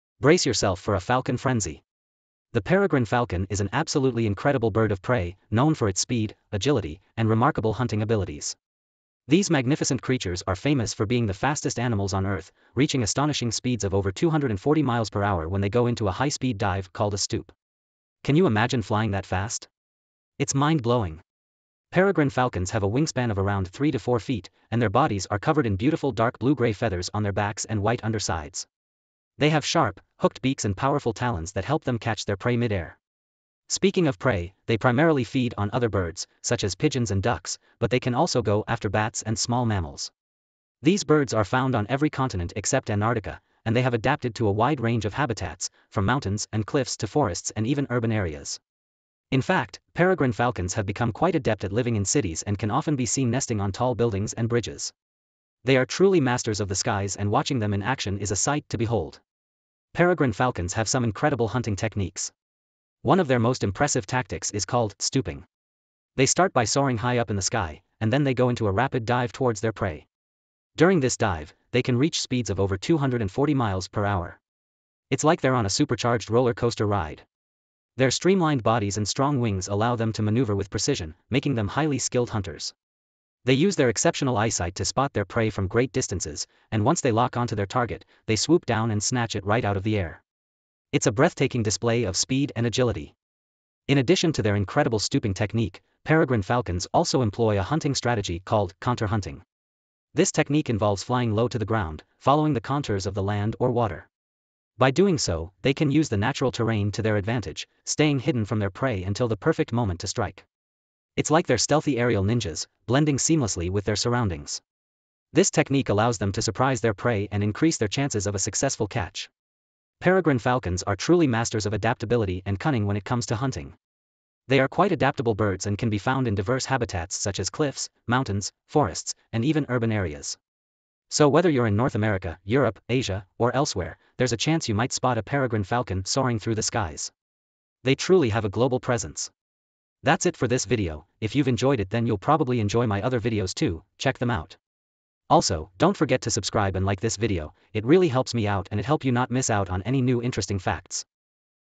Facts YOU SHOULD KNOW about the PEREGRINE FALCON
Brace yourself for a falcon frenzy! The peregrine falcon is an absolutely incredible bird of prey, known for its speed, agility, and remarkable hunting abilities. These magnificent creatures are famous for being the fastest animals on Earth, reaching astonishing speeds of over 240 miles per hour when they go into a high-speed dive called a stoop. Can you imagine flying that fast? It's mind-blowing! Peregrine falcons have a wingspan of around 3 to 4 feet, and their bodies are covered in beautiful dark blue-gray feathers on their backs and white undersides. They have sharp, hooked beaks and powerful talons that help them catch their prey mid-air. Speaking of prey, they primarily feed on other birds, such as pigeons and ducks, but they can also go after bats and small mammals. These birds are found on every continent except Antarctica, and they have adapted to a wide range of habitats, from mountains and cliffs to forests and even urban areas. In fact, peregrine falcons have become quite adept at living in cities and can often be seen nesting on tall buildings and bridges. They are truly masters of the skies and watching them in action is a sight to behold!

Peregrine falcons have some incredible hunting techniques. One of their most impressive tactics is called "stooping." They start by soaring high up in the sky, and then they go into a rapid dive towards their prey. During this dive, they can reach speeds of over 240 miles per hour! It's like they're on a supercharged roller coaster ride! Their streamlined bodies and strong wings allow them to maneuver with precision, making them highly skilled hunters. They use their exceptional eyesight to spot their prey from great distances, and once they lock onto their target, they swoop down and snatch it right out of the air. It's a breathtaking display of speed and agility!

In addition to their incredible stooping technique, peregrine falcons also employ a hunting strategy called "contour-hunting." This technique involves flying low to the ground, following the contours of the land or water. By doing so, they can use the natural terrain to their advantage, staying hidden from their prey until the perfect moment to strike. It's like they're stealthy aerial ninjas, blending seamlessly with their surroundings. This technique allows them to surprise their prey and increase their chances of a successful catch. Peregrine falcons are truly masters of adaptability and cunning when it comes to hunting!

They are quite adaptable birds and can be found in diverse habitats such as cliffs, mountains, forests, and even urban areas. So whether you're in North America, Europe, Asia, or elsewhere, there's a chance you might spot a peregrine falcon soaring through the skies! They truly have a global presence.

That's it for this video, if you've enjoyed it then you'll probably enjoy my other videos too, check them out!